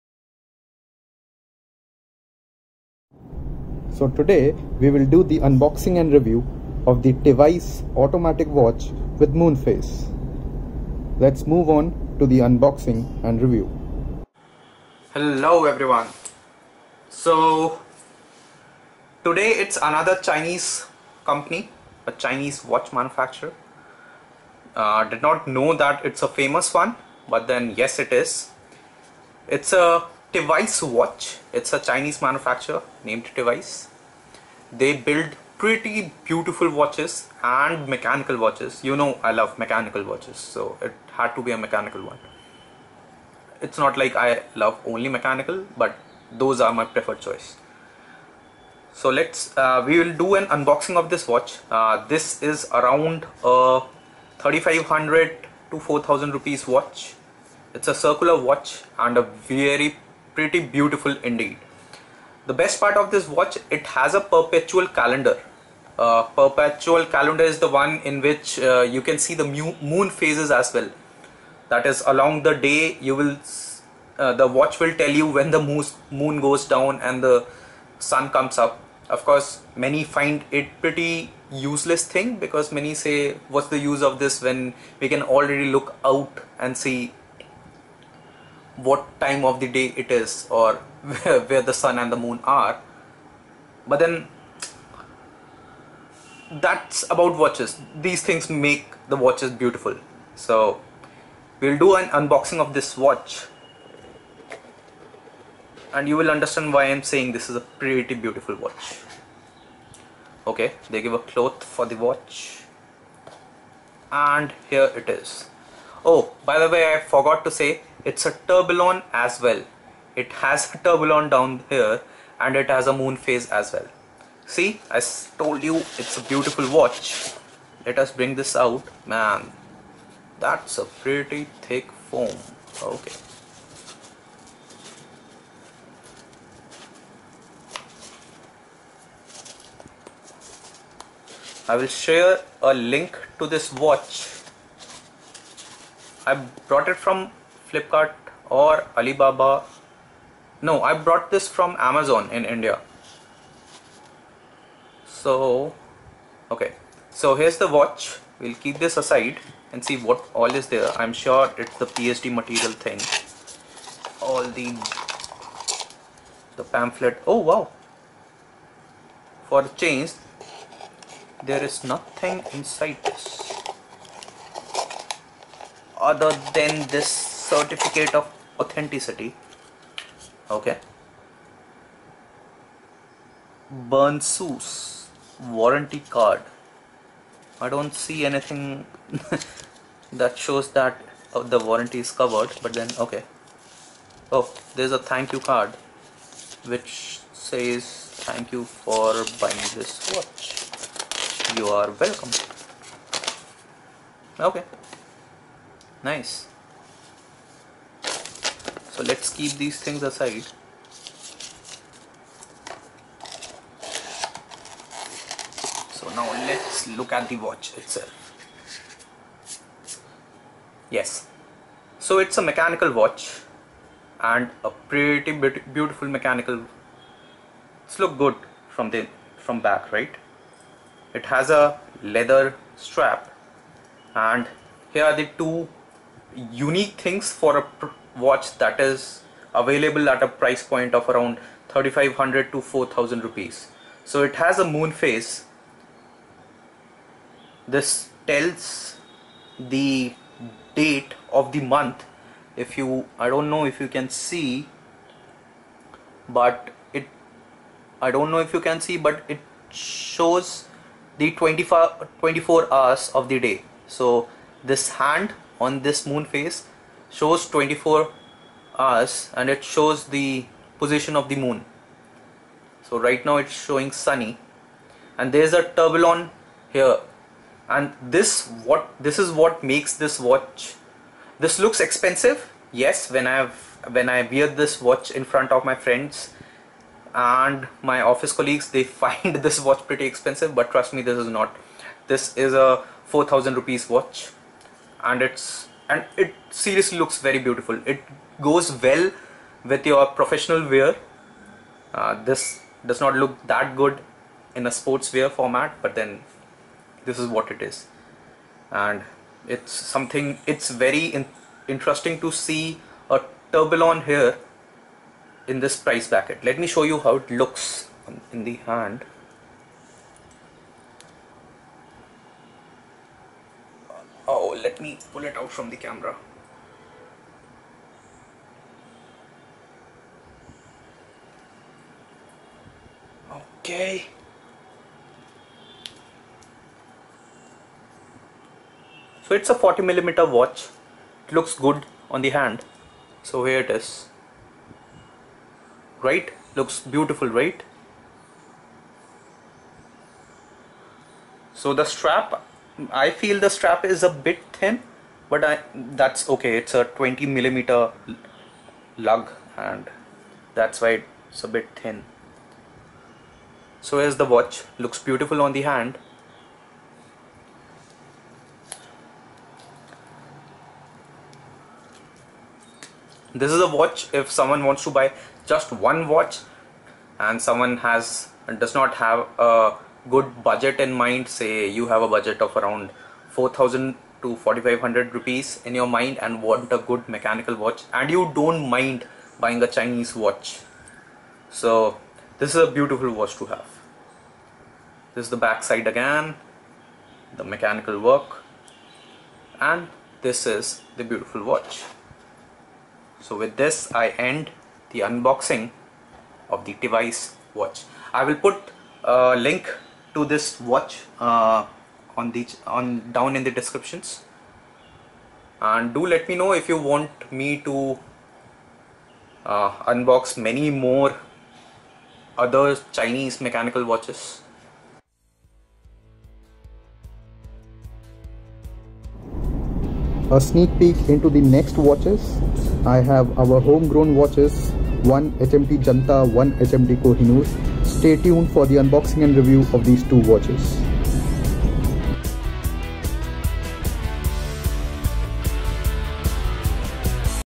Tevise T612 Automatic - Unboxing and Review - Affordable and Classy automatic watch in India!
The watch is a Chinese automatic watch. It has a good mechanical movement giving a power reserve for 1.5 - 2 days, which is unexpected from a Chinese movement.

Best for professional and casual wear.

This watch costs around INR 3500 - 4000, depending upon the offers available at any time of the year.

PS: I am against promoting Chinese products. Since, I already owned this watch, so though of unboxing.